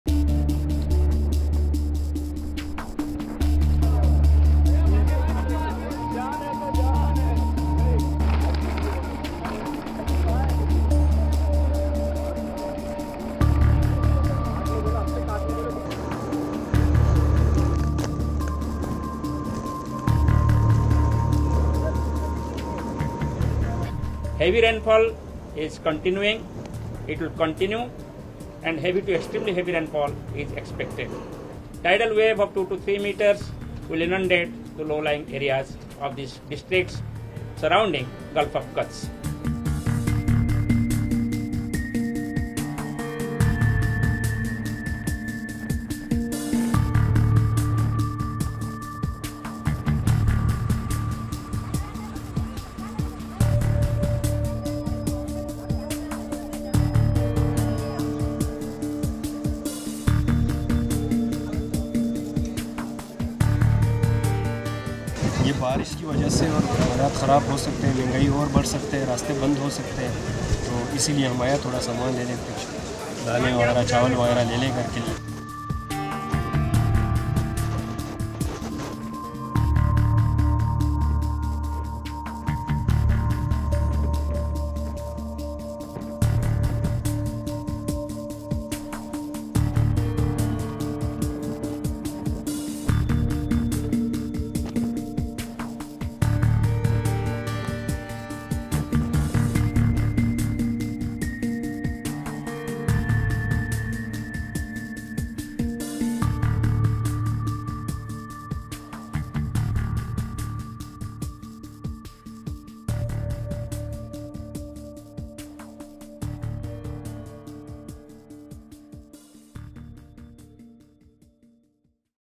Cyclone Biparjoy looms over India and Pakistan as thousands evacuated
Landslides, extreme winds and heavy rain have been battering western India's Gujarat coast and neighbouring southern Pakistan as cyclone Biparjoy nears land. More than 180,000 people in India and Pakistan have been evacuated ahead of the powerful storm, whose name means “disaster” or “calamity” in Bengali.